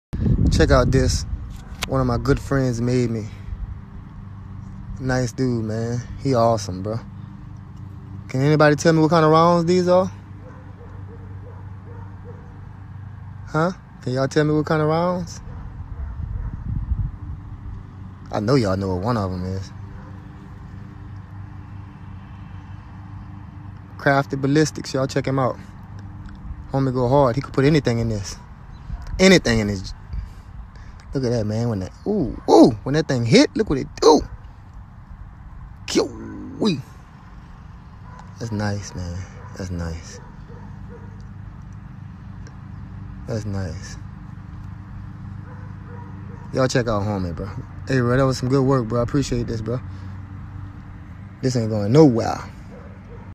Black Rambo Tv giving a shoutout
Here a custom piece made for Black Rambo TV. A 9mm, a 5.7 and a 50AE. His favorite calibers. Thanks again bro!